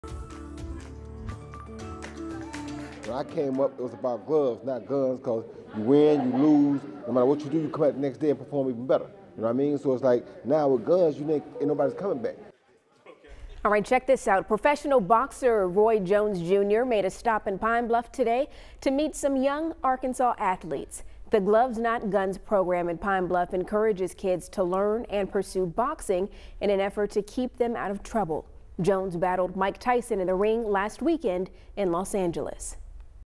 Roy Jones Jr. visits Pine Bluff to meet young boxers
Following his fight with Mike Tyson, Roy Jones Jr. made a trip to Pine Bluff to meet with some young boxers that are a part of the Gloves Not Guns program.